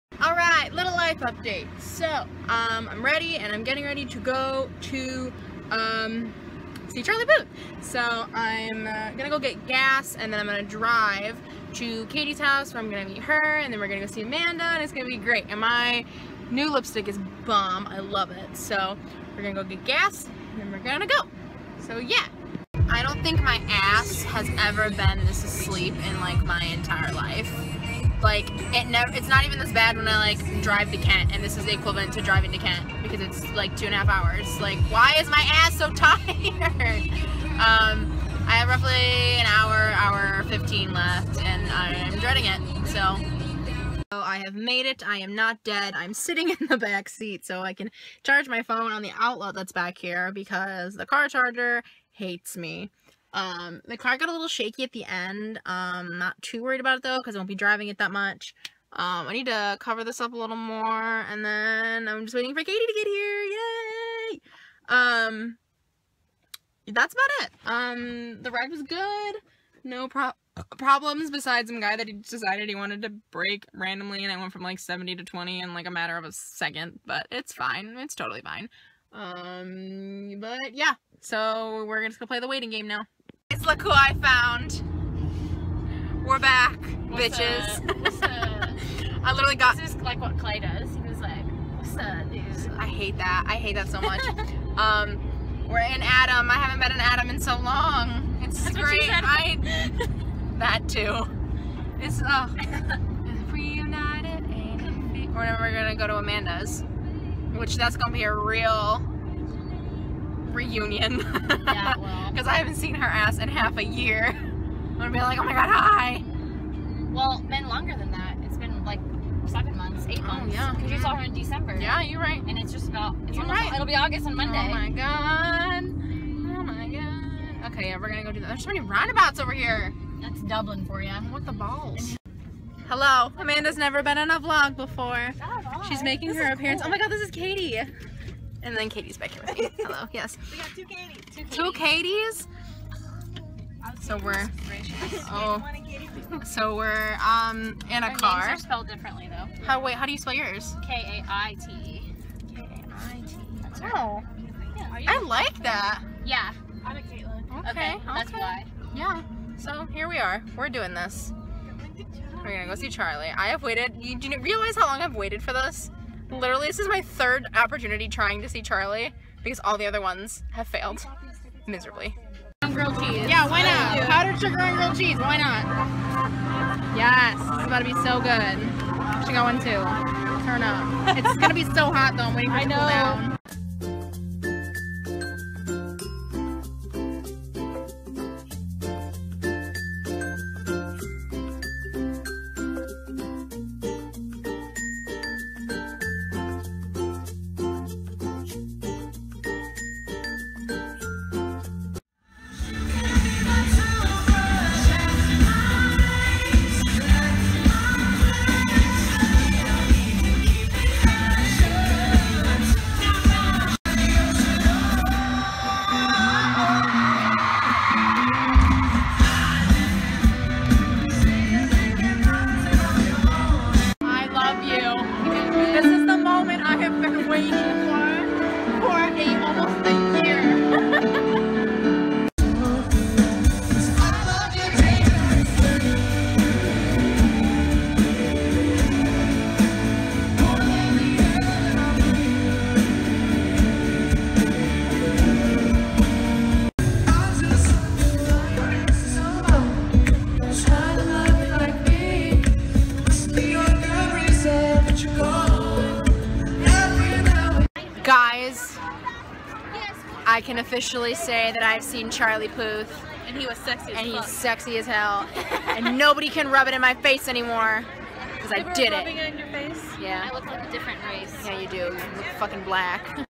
Charlie (July 29, 2016)
Today I finally get to see Charlie Puth!!!